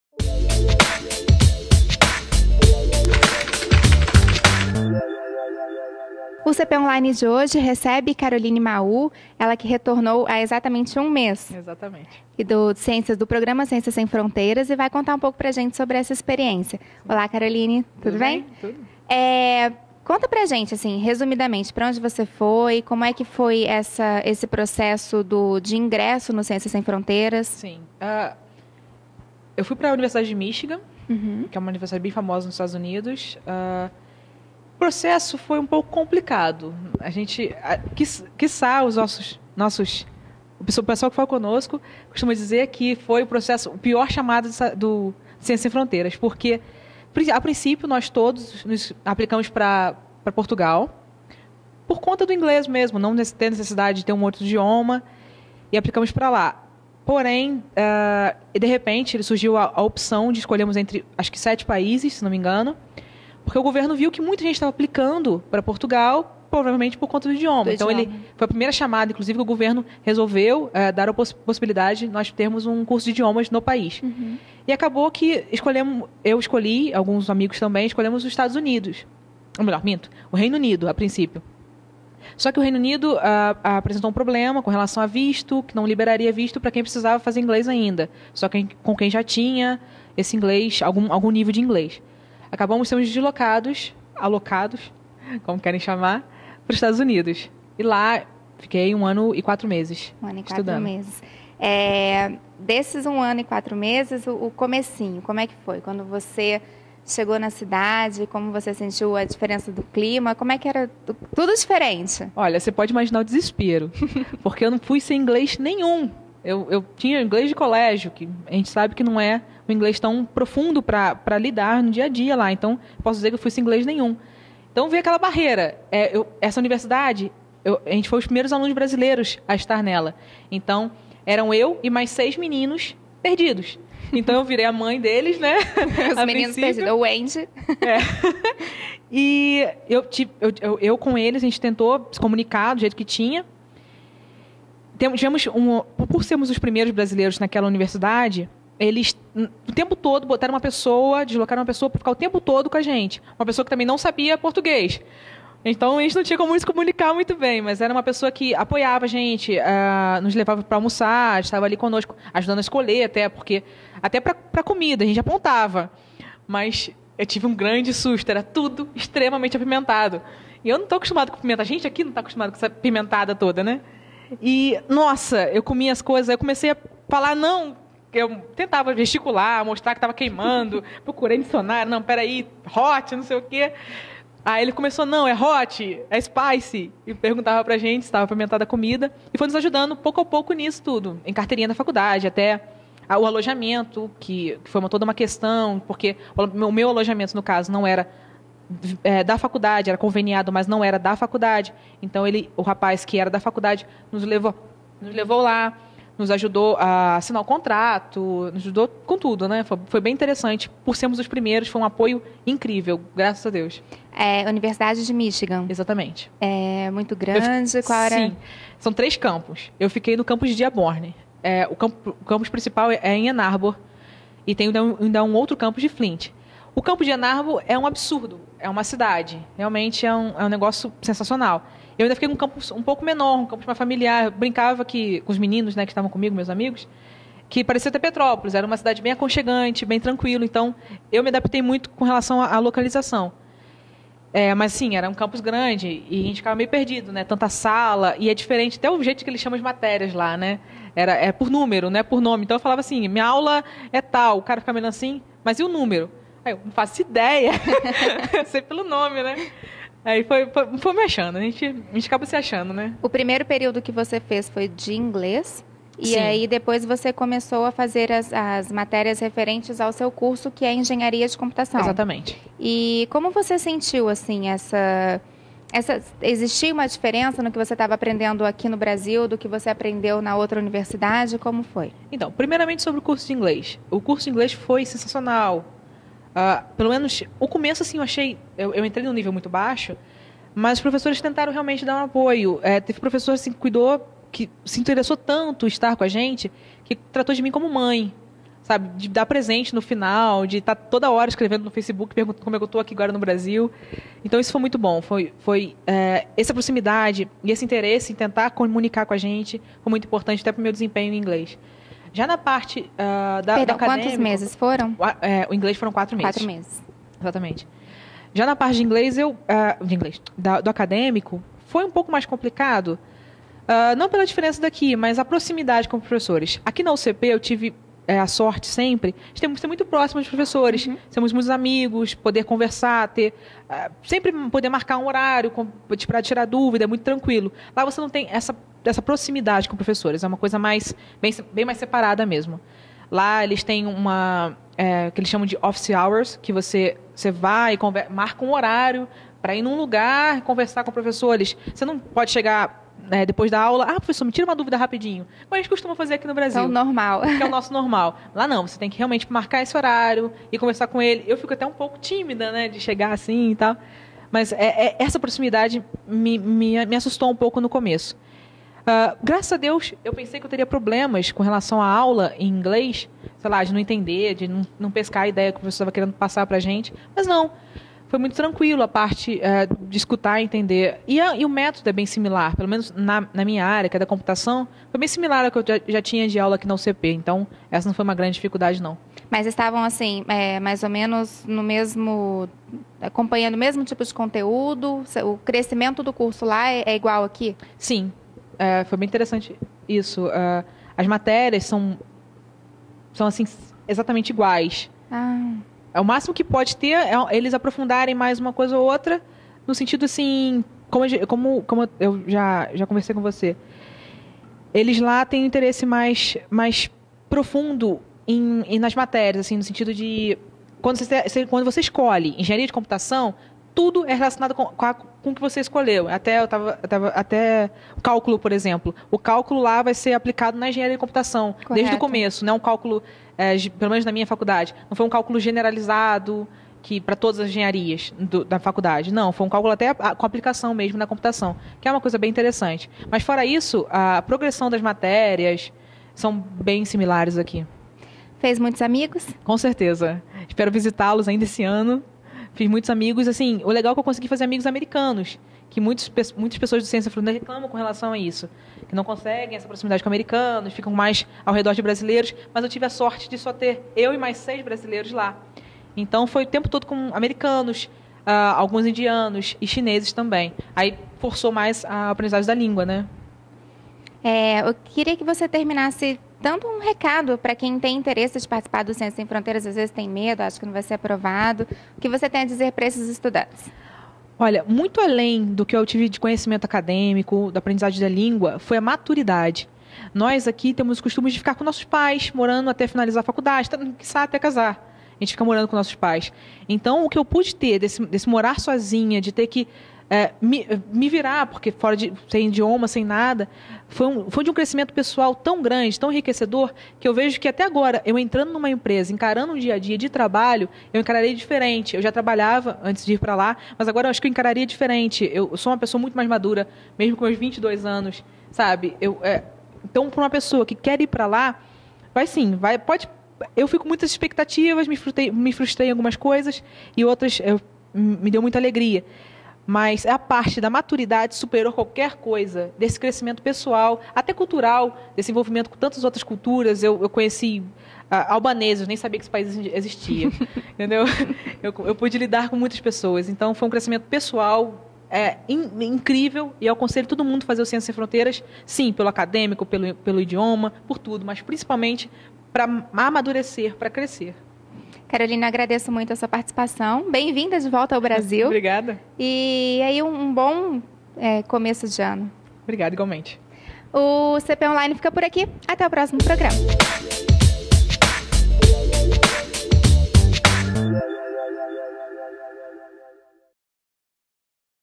Ciência sem Fronteiras na UCP: entrevista com a aluna Caroline Maul
Foco: a aluna da UCP Carolina Maul conta como foi participar do programa Ciência sem Fronteiras. Assista!